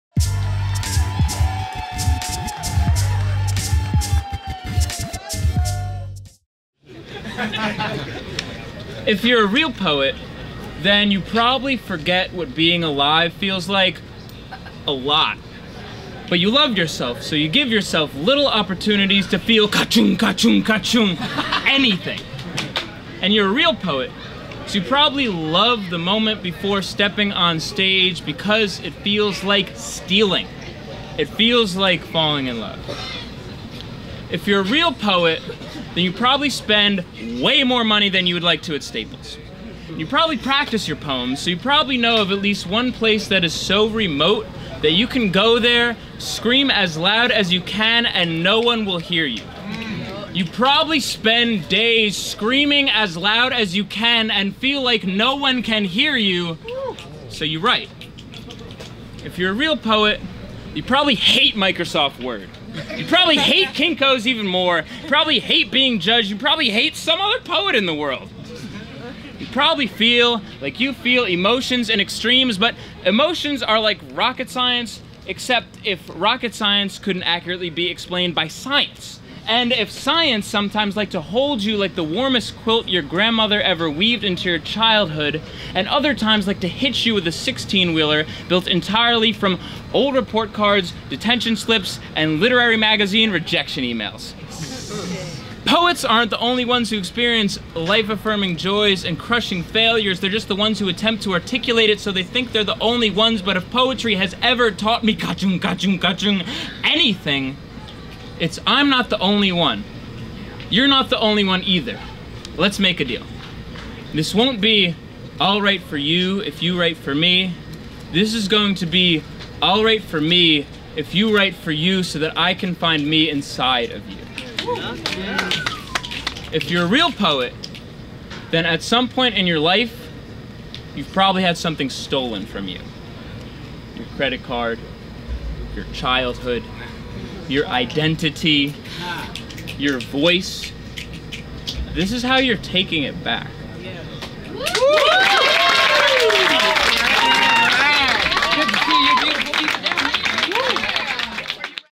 Scott Sigl - "If You're A Real Poet" @WANPOETRY (UNOFFICIAL NPS CYPHER 2016)
Scott Sigl performing "If You're A Real Poet" at the Cherokee Man and the Mockingbird graffiti mural at Decatur Station in Decatur, Georgia.